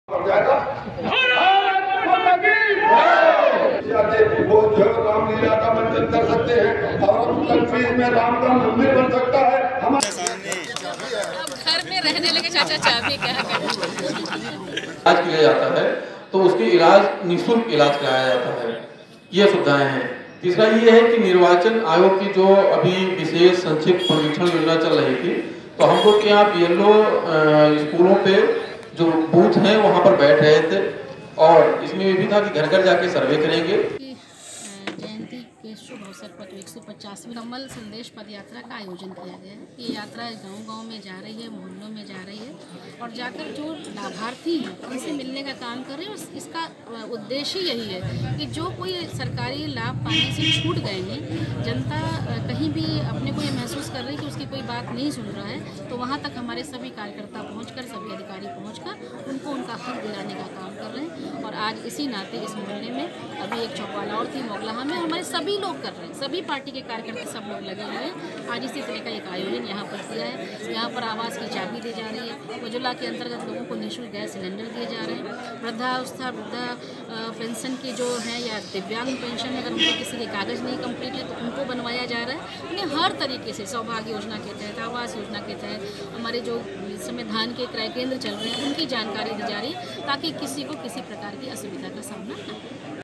बहराइच: चौपाल लगाकर सरकारी योजनाओं की दी जानकारी
बहराइच: राष्ट्रपिता महात्मा गांधी की 150वीं जयंती के मौके पर भाजपा की ओर से कमल संदेश यात्रा निकाली जा रही है. इसी क्रम में मंगलवार की रात अनुपमा जायसवाल ने शहर के मोहल्ला गुदड़ी स्थित ओमकार धर्मशाला के पास चौपाल लगाई.  इसमें उन्होंने अधिकारियों के जरिये सरकार की ओर से चलाई जा रही कल्याणकारी योजनाओं की जानकारी दी. इस अवसर पर मंत्री अनुपमा जायसवाल नें प्रधानमंत्री आवास योजना के पात्रों आवास की चाभी सौंपी.